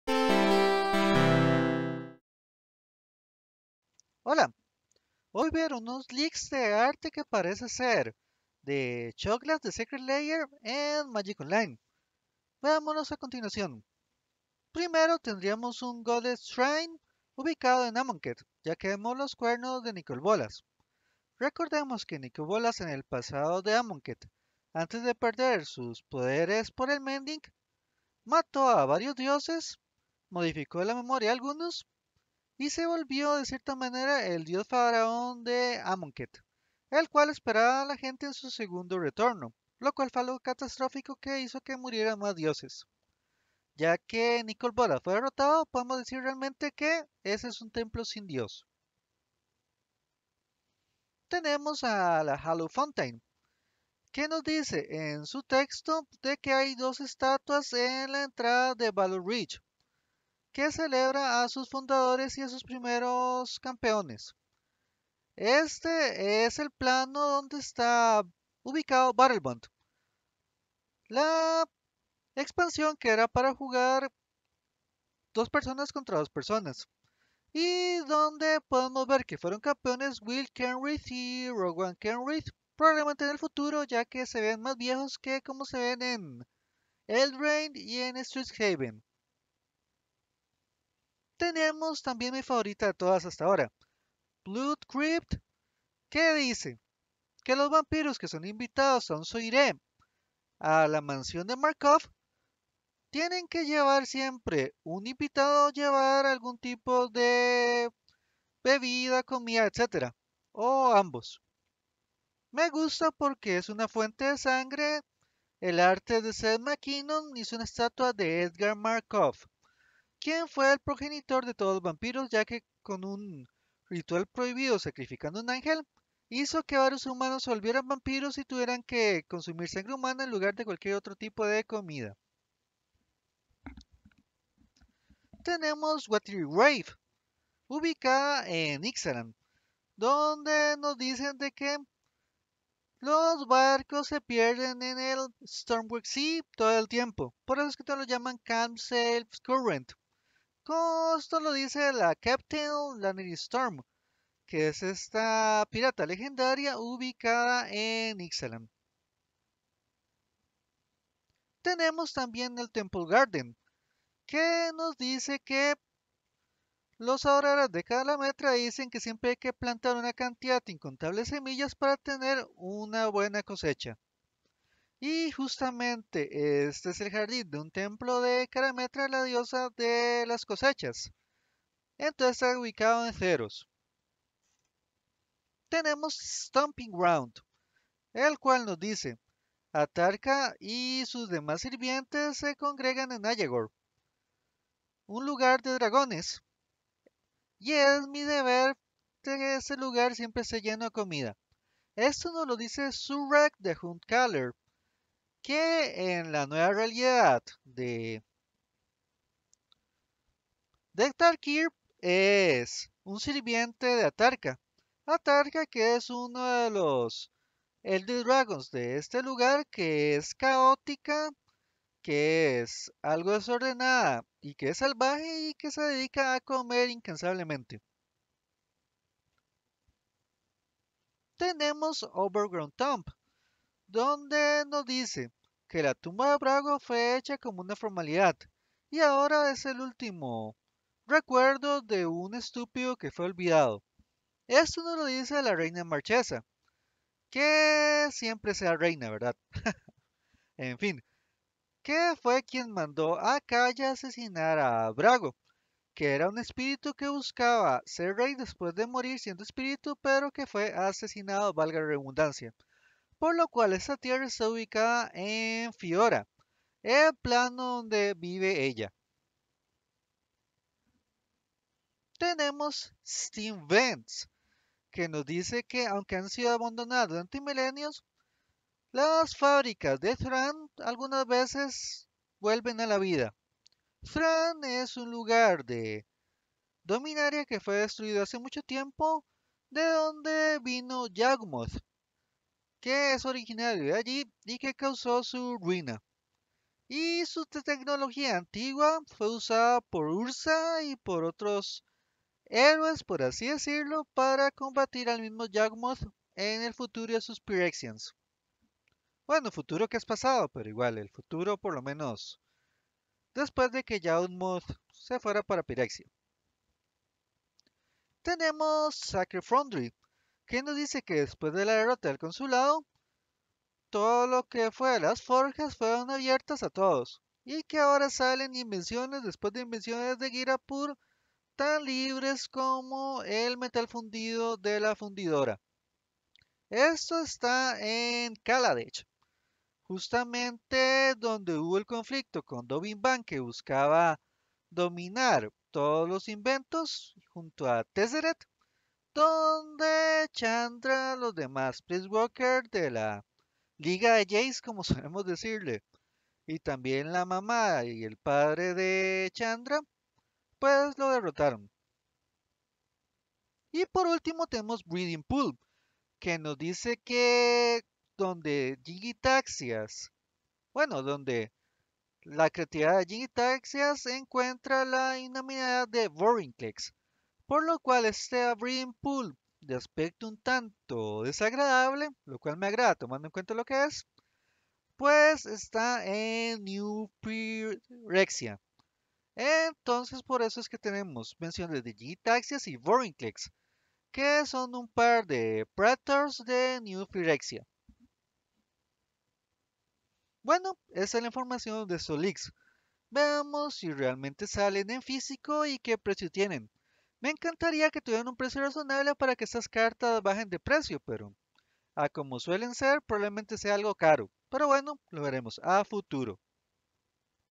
¿Secret Lair de Shocklands?
Hola. Hoy aparecieron en Magic Online artes alternos de las 10 shocklands, ubicados en diferentes planos. Veámoslos a continuación.